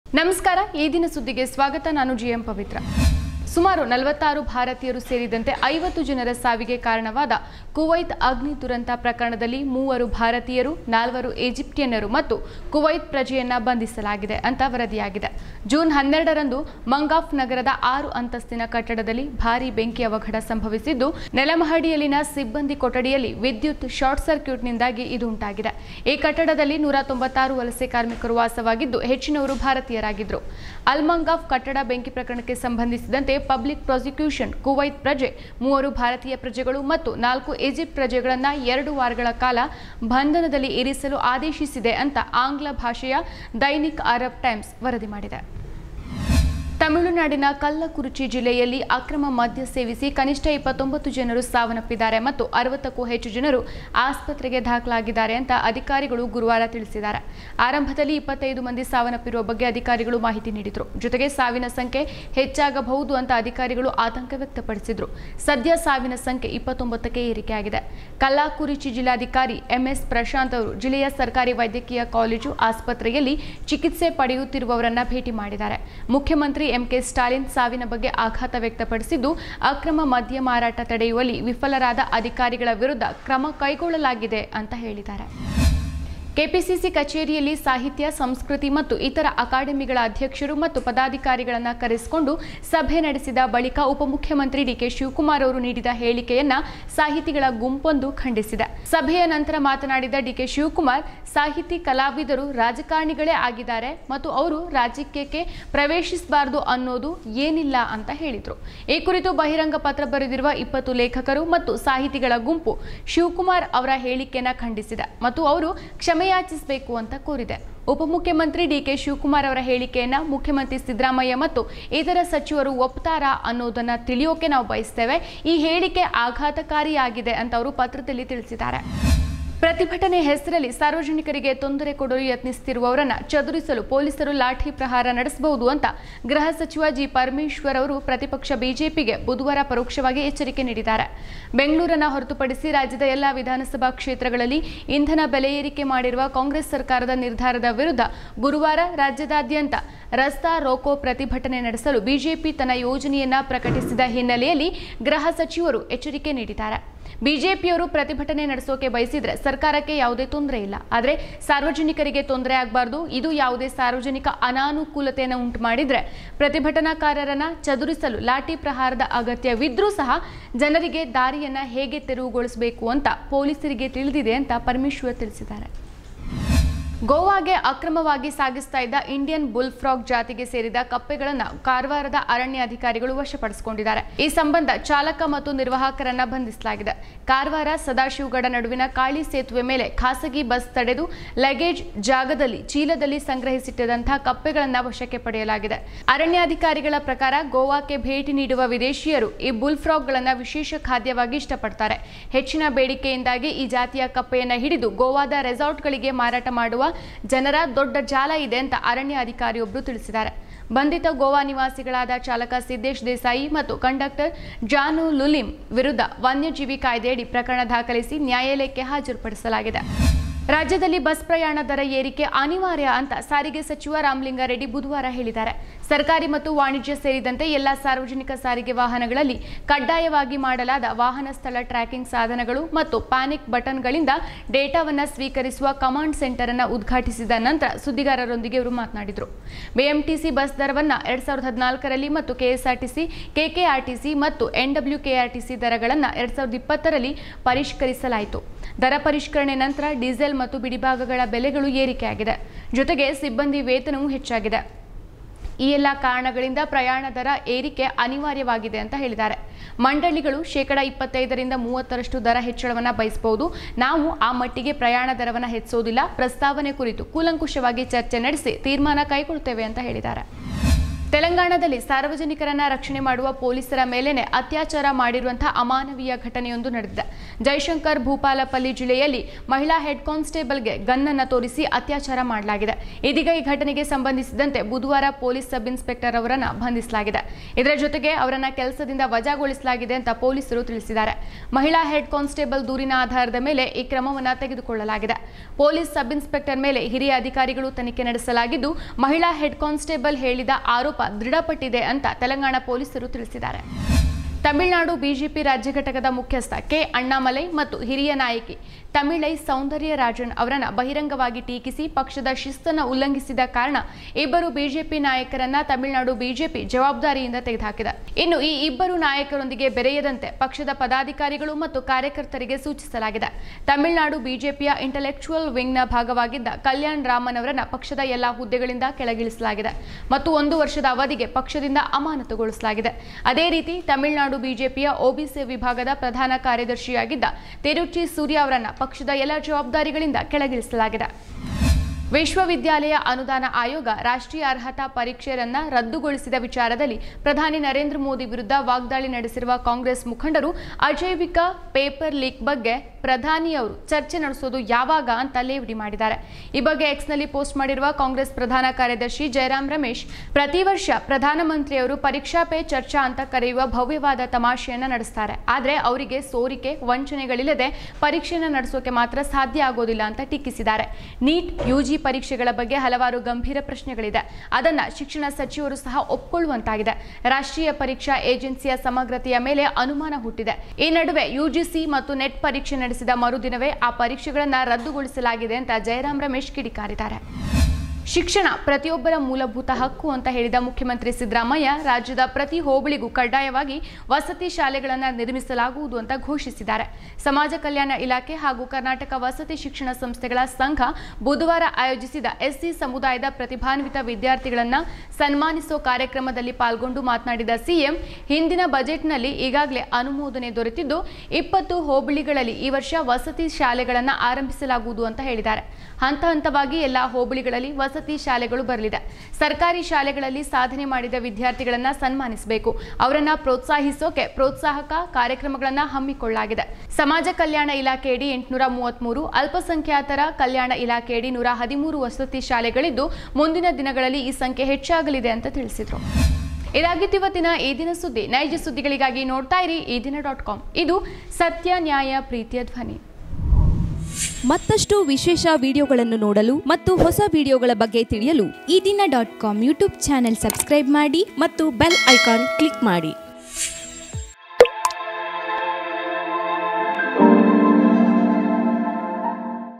ಈ ದಿನದ ರಾಜ್ಯದ, ರಾಷ್ಟ್ರದ, ಮತ್ತು ಅಂತಾರಾಷ್ಟ್ರೀಯ ಪ್ರಮುಖ ಸುದ್ಧಿಗಳು
ಕುವೈತ್ ಅಗ್ನಿ ದುರಂತ I ಮೂವರು ಭಾರತೀಯರು, ನಾಲ್ವರು ಈಜಿಪ್ಟ್ ಪ್ರಜೆಗಳ ಬಂಧನ !
ತಮಿಳುನಾಡು I ಅಕ್ರಮ ಮದ್ಯ ಸೇವಿಸಿ 29 ಸಾವು !
ಡಿಕೆ ಶಿವಕುಮಾರ್ ಹೇಳಿಕೆಗೆ ಖಂಡನೆ; ಕ್ಷಮೆಯಾಚಿಸುವಂತೆ ಸಾಹಿತಿಗಳ ಬಹಿರಂಗ ಪತ್ರ !
ಪ್ರತಿಭಟನೆ ಹೆಸರಿನಲ್ಲಿ ಜನರಿಗೆ ತೊಂದರೆ ನೀಡಿದರೆ ಲಾಠಿ ಪ್ರಹಾರಕ್ಕೂ ಹಿಂಜರಿಯಲ್ಲ;
ಬಸ್ ಪ್ರಯಾಣ ದರ ಏರಿಕೆ ಅನಿವಾರ್ಯ; ಸಾರಿಗೆ ಸಚಿವ ರಾಮಲಿಂಗಾರೆಡ್ಡಿ !